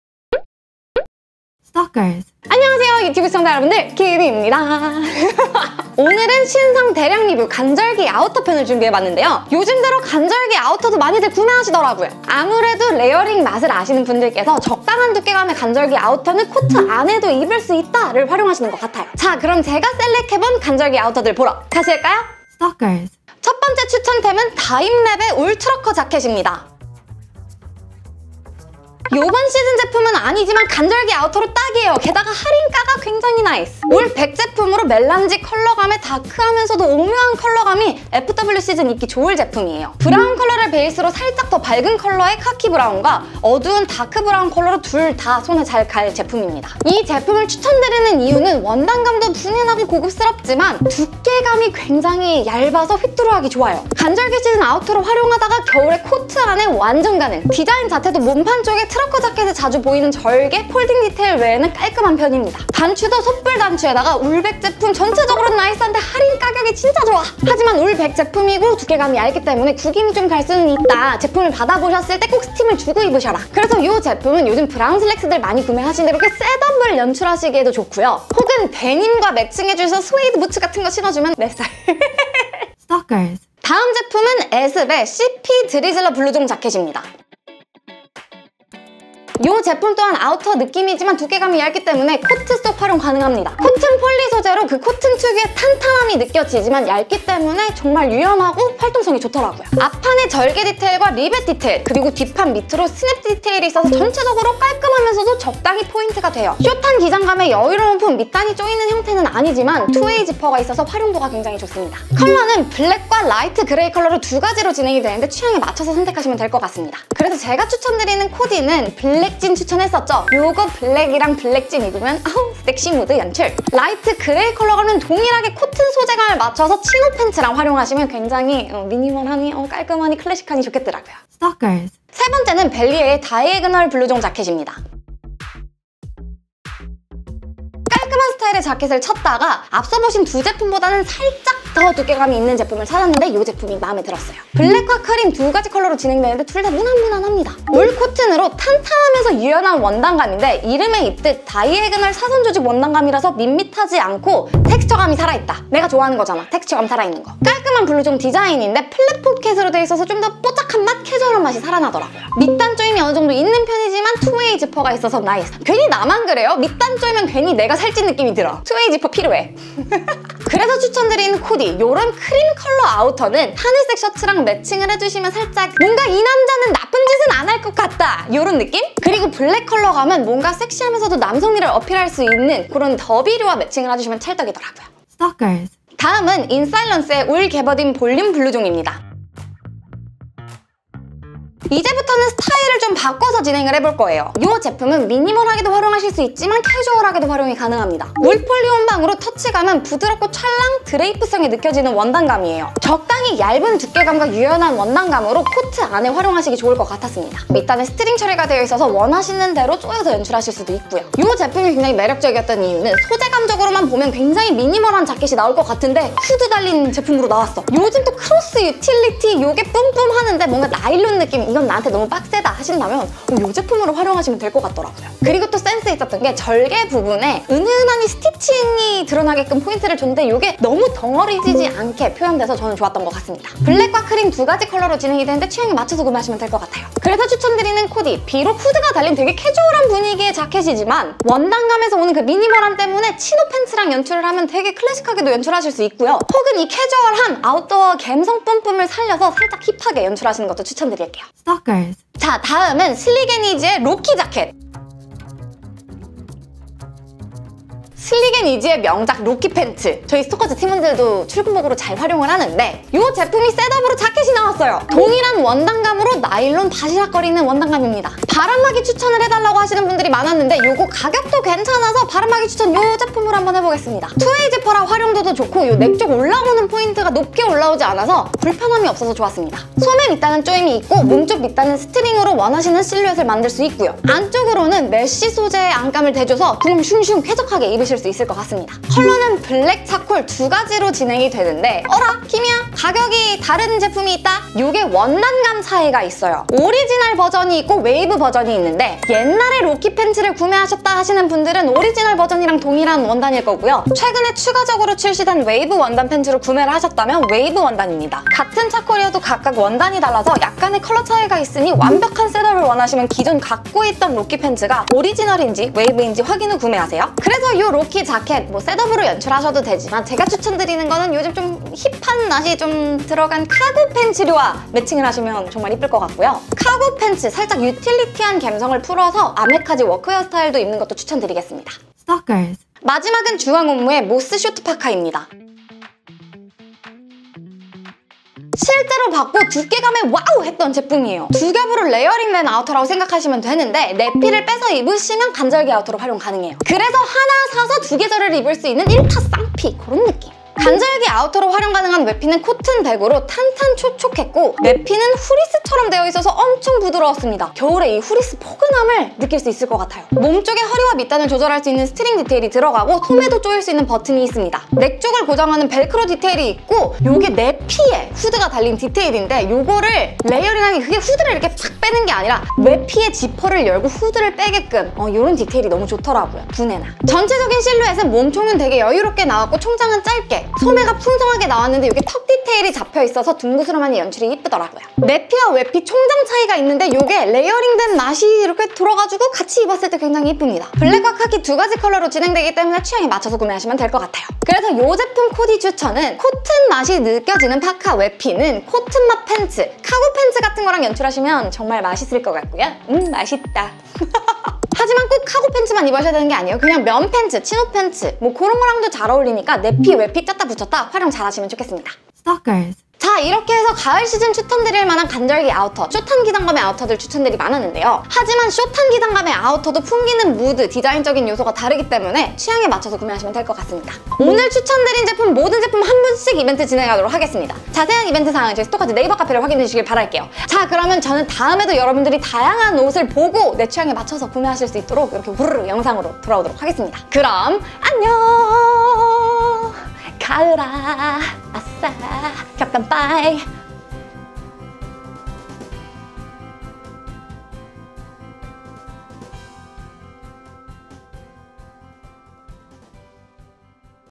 6만원~30만원대 예쁜 가을 아우터 6종 추천
**오늘 영상은 제가 한 제품, 한 제품 셀렉해서 브랜드 요청을 드려서,
   금전지원 없이 제품 지원만 받아서 만들어졌습니다**
*영상 속 코디 아이템의 정보는 아래 링크를 참고해주세요!

오늘은 간절기에 가볍게 걸치기 좋은 가을 아우터 영상을 준비했습니다!
다양한 가격대와 스타일의 숏자켓들이니 오늘 준비한 아이템들 중
여러분의 취향을 저격하는 제품이 하나쯤이 있으셨으면 좋겠습니다🙏

(5) [슬릭앤이지] Rocky Jacket Black 로키재킷 OG
**차콜컬러의 경우 29일 판매 업로드 예정입니다**

0:00 오프닝
0:23 [다임 랩] WOOL TRUCKER JACKET
1:23 [애습] CP 드리즐러 블루종 자켓
2:11 [벨리에] 다이에그널 블루종 자켓
3:05 [인사일런스] 울 개버딘 볼륨 블루종
4:11 [슬릭앤이지] Rocky Jacket
5:32 [주앙옴므] MODS SHORT PARKA
6:52 엔딩